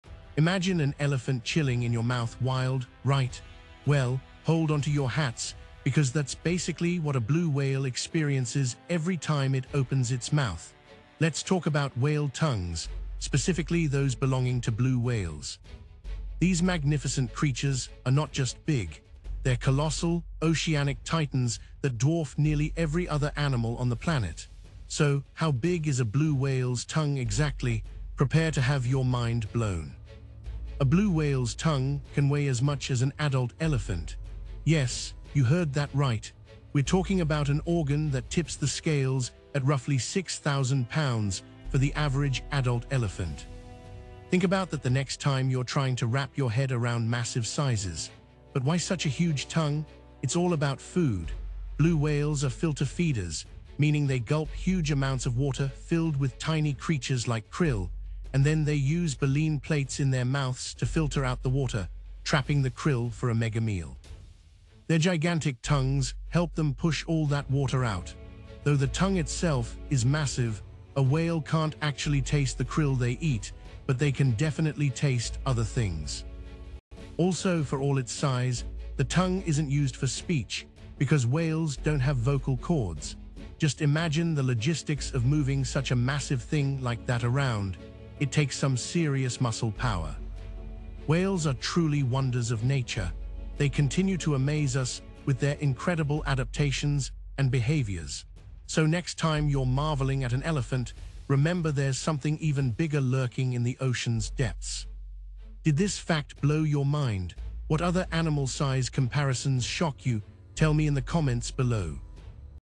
Whale Tongue vs. Elephant: Mind-Blowing Size Comparison!
Dive into the incredible facts about blue whales! You won't believe the weight of their tongues!